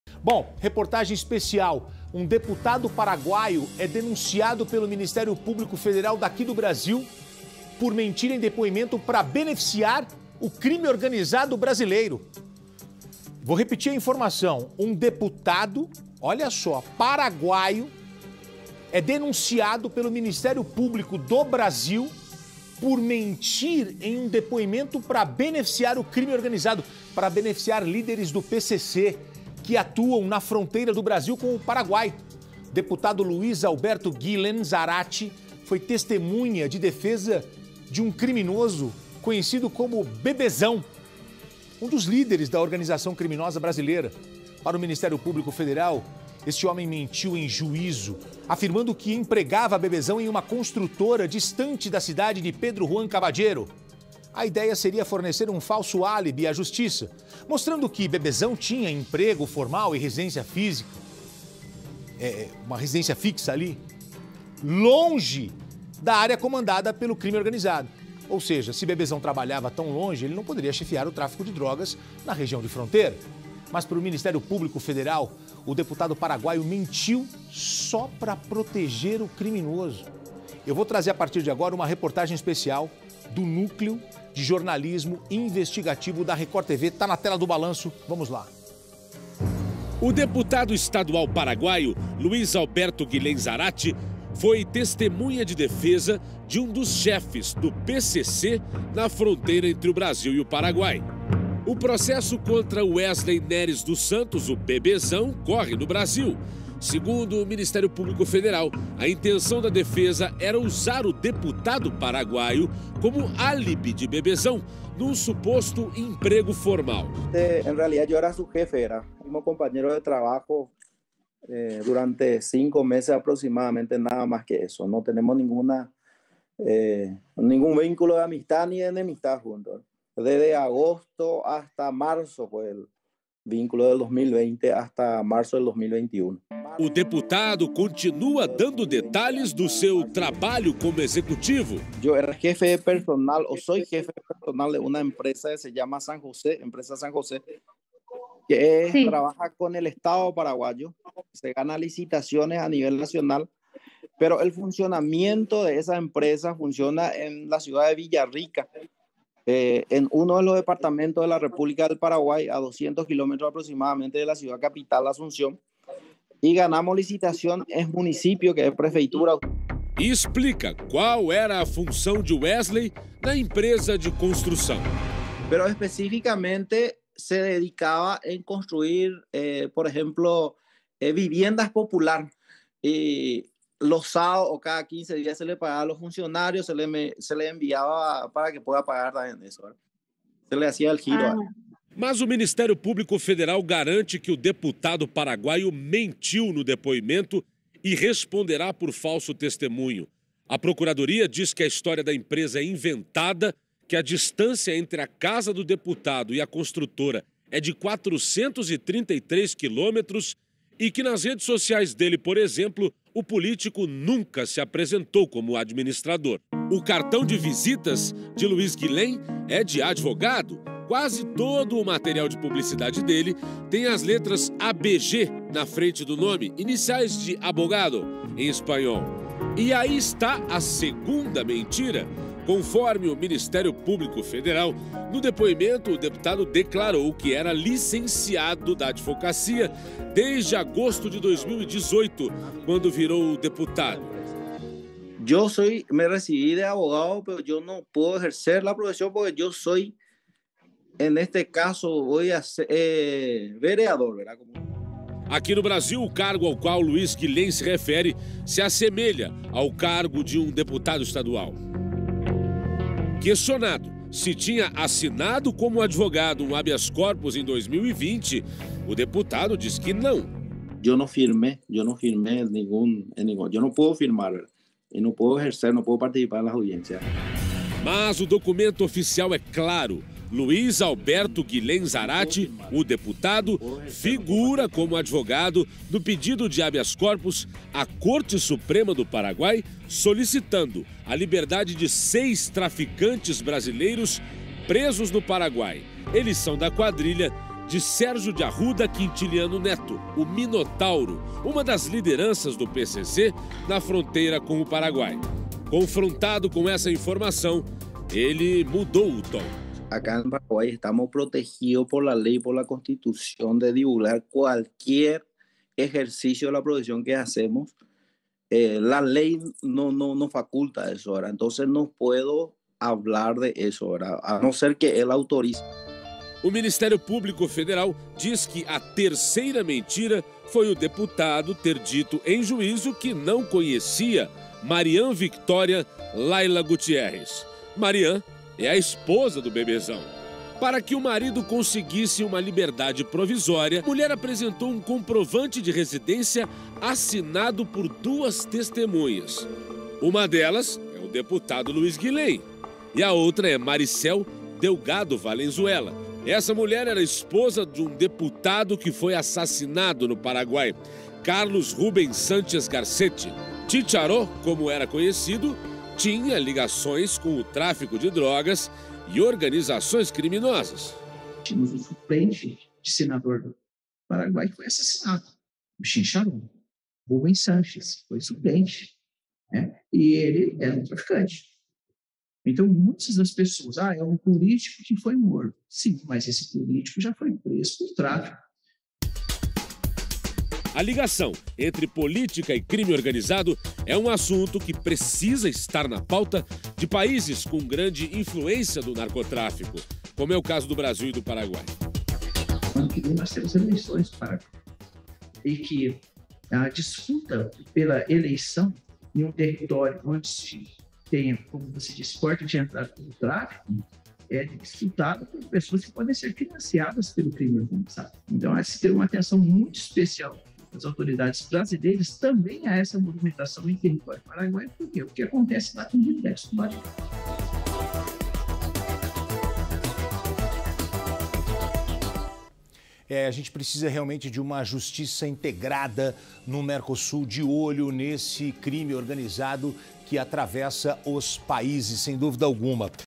Deputado paraguaio mentiu para beneficiar líderes do PCC, diz MP
Um deputado paraguaio foi denunciado pelo Ministério Público Federal por mentir em depoimento para beneficiar líderes do PCC, que atuam na fronteira do Brasil com Paraguai. Luís Alberto Guillén Zaráte foi testemunha de defesa do criminoso "Bebezão", um dos líderes da organização criminosa brasileira. Para o Ministério Público Federal, Zaráte mentiu em juízo afirmando que empregava "Bebezão" em uma construtora distante da cidade de Pedro Juan Caballero.